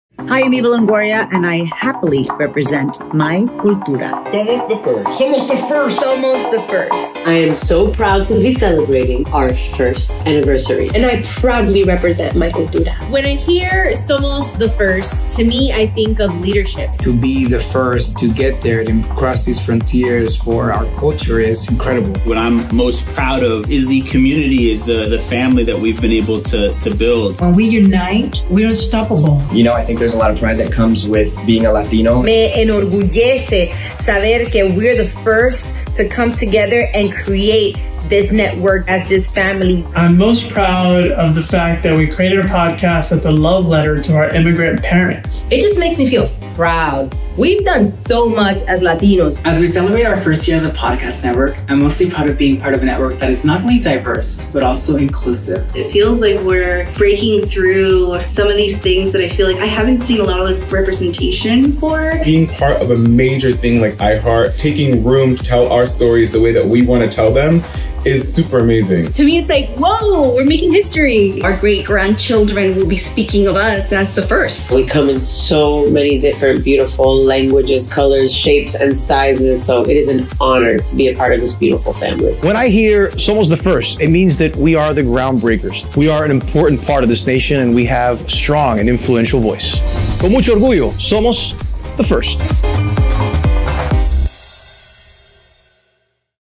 Somos the First! My Cultura celebrates one year!
My Cultura Podcast Network celebrates its first anniversary! Share in the excitement as our talented podcast hosts, from diverse Latinx backgrounds, commemorate all our firsts, including being the first Latinx Podcast Network at iHeartMedia. We’re all about educating and elevating our voices, creating podcasts for everyone. Join in the celebration of Somos the first – We are the first!